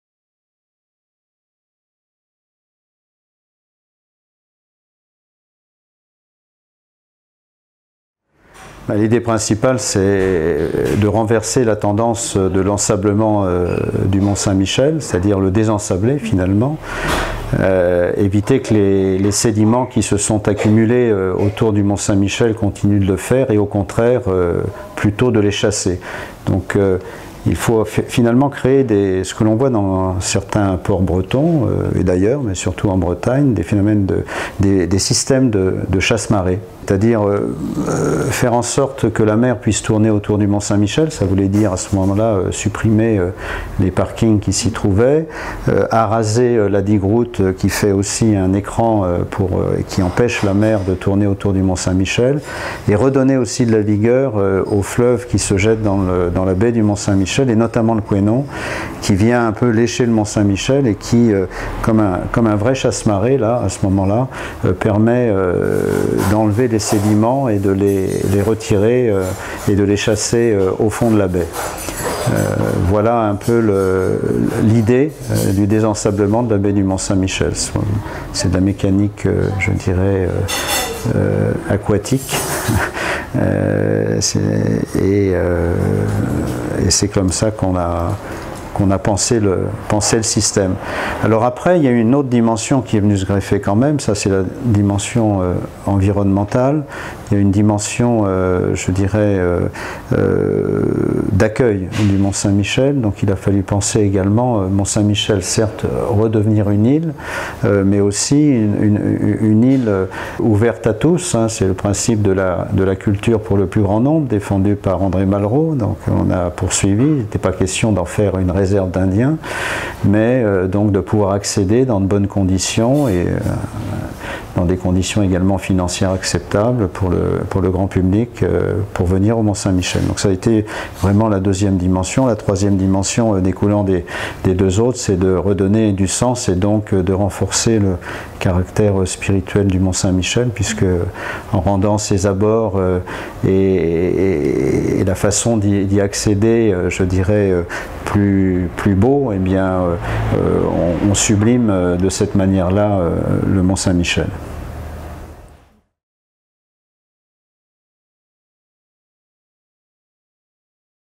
ОСНОВЫЕ НАПРАВЛЕНИЯ ПРОЕКТА - ЭРИК ВАНЬЕ - ПОСЕЩЕНИЕ МОН-СЕН-МИШЕЛЯ - 3
ОСНОВЫЕ НАПРАВЛЕНИЯ ПРОЕКТА - ЭРИК ВАНЬЕ - ПОСЕЩЕНИЕ МОН-СЕН-МИШЕЛЯ

С точки зрения развития туризма и экономики, каковы основные вызовы, которые нужно решить для того, чтобы остров оставался любимым памятником для французских и иностранных туристов?
Реализация: Mediaventilo
Ресторан: Le Petit Rétro – Paris
Проект по восстановлению островной природы Мон-Сен-Мишеля